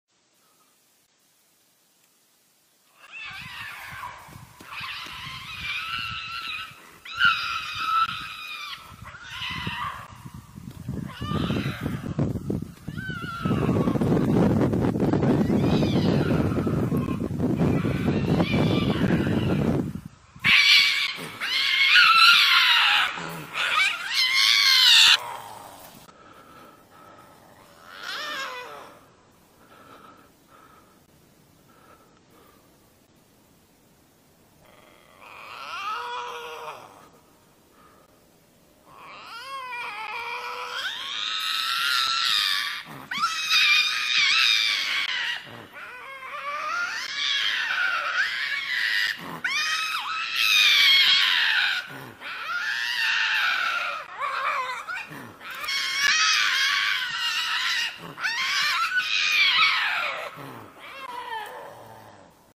Male lynx chasing female up a tree in Alberta, Canada
Want to support me?